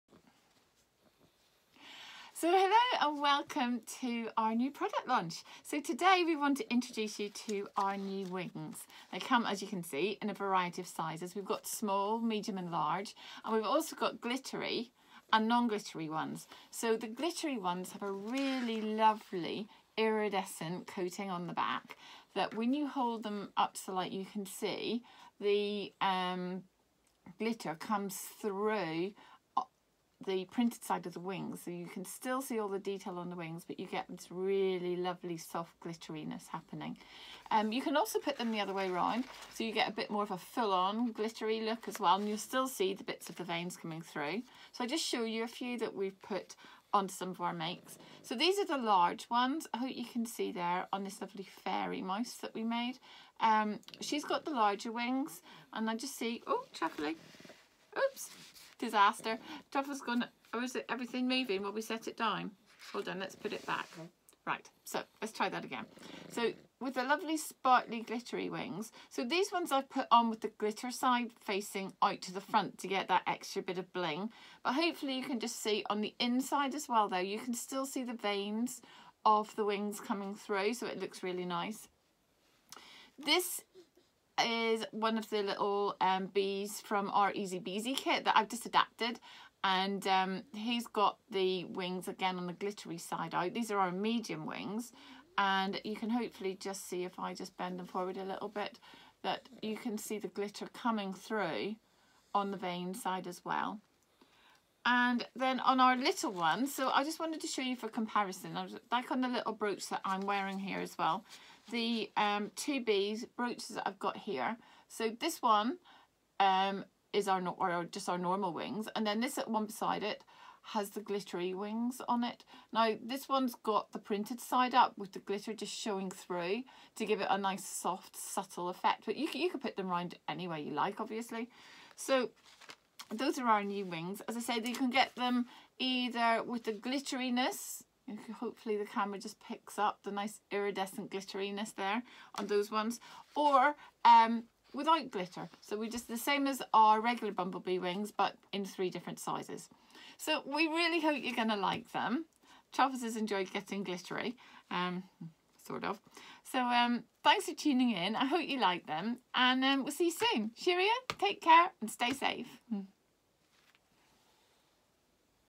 Assorted (sparkly) wings
Truffles and Alison introduce our assorted size wings- with and without added sparkles!